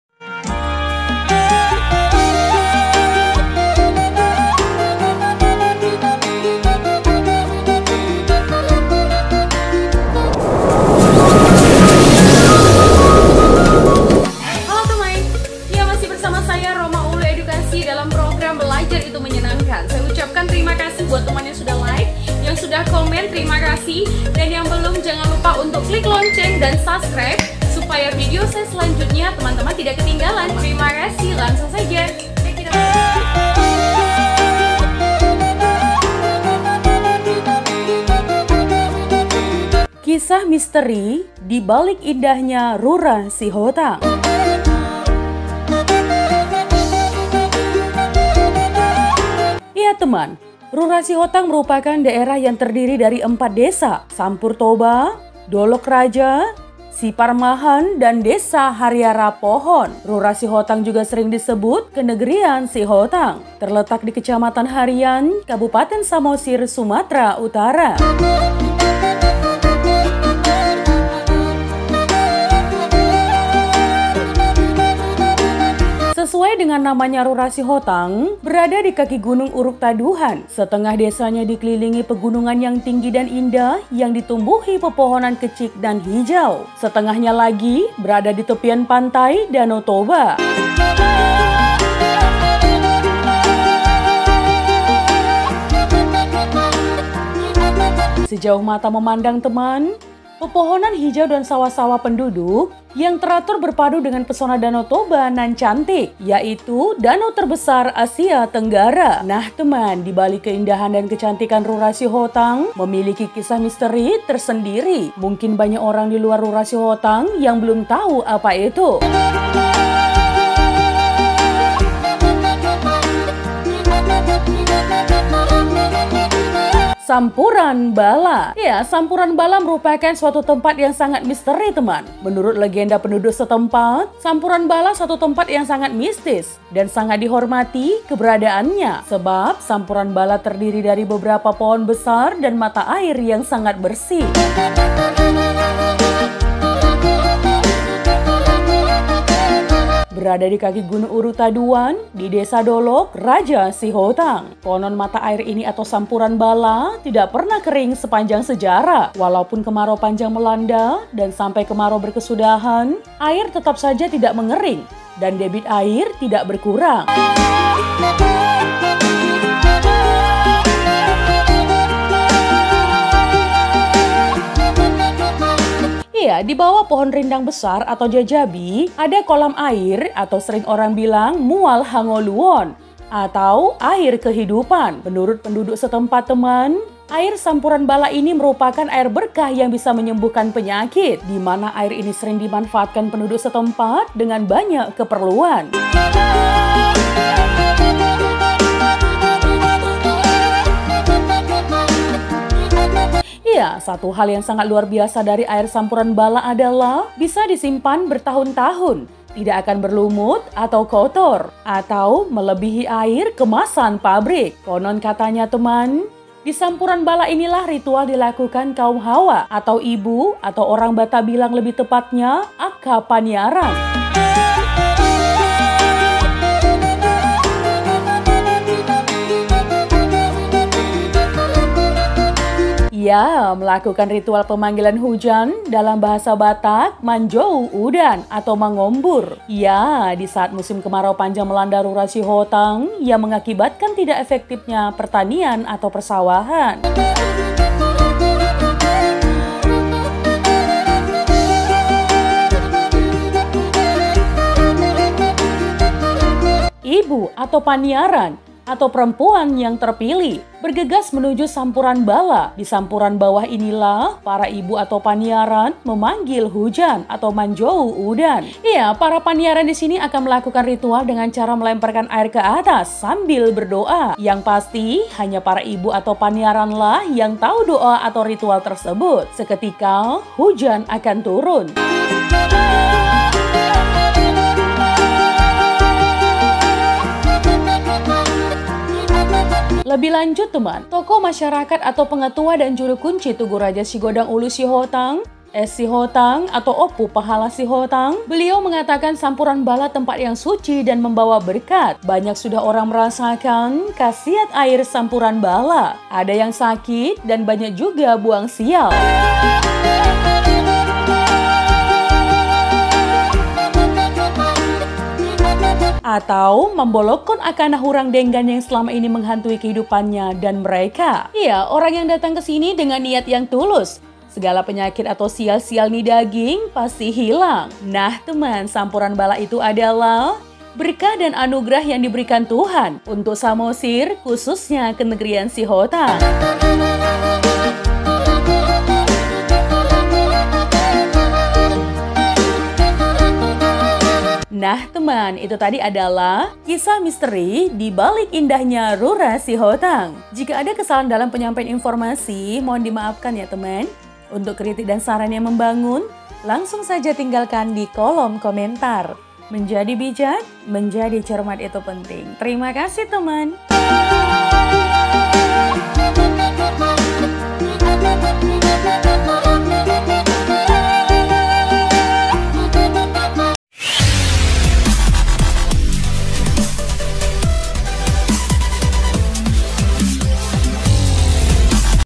Indahnya RURA SIHOTANG Memiliki Kisah MISTERI
Rura Sihotang merupakan daerah yang terdiri dari empat (4) Desa.SAMPUR TOBA, DOLOK RAJA, SIPARMAHAN dan Desa HARIARA POHAN, Rura Sihotang juga sering disebut Kenegerian Sihotang. Terletak di kecamatan Harian Kabupaten Samosir, Sumatera utara. Sesuai dengan namanaya Rura Sihotang berada di kaki gunung Uruk Taduhan